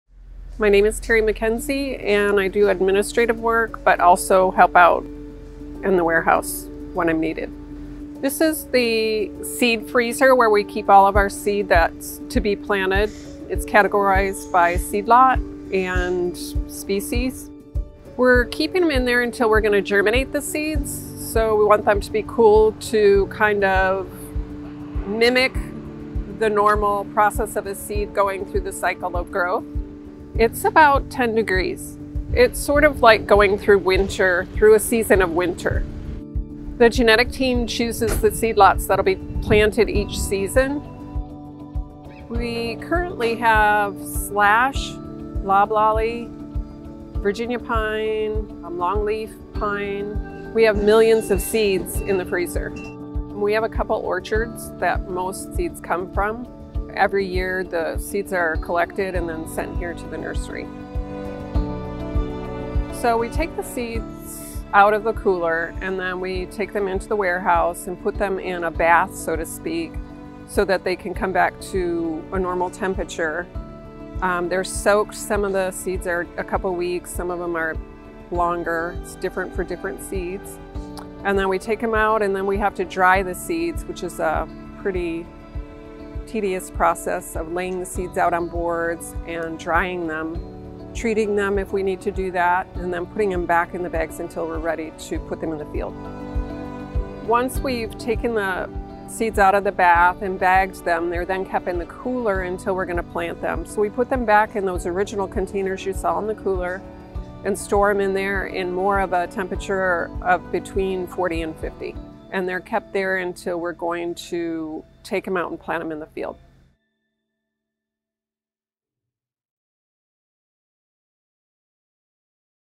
Tour Stop 2: The Seed Freezer
Welcome to stop 2 on our tour of Rayonier's pine tree seedling nursery in Elberta, Alabama! We keep millions of seeds in our very cold walk-in freezer. This simulates cold weather, which causes them to go dormant until we warm and rehydrate them for replanting. This is video #2 of our 11-video tour. Thanks for joining us!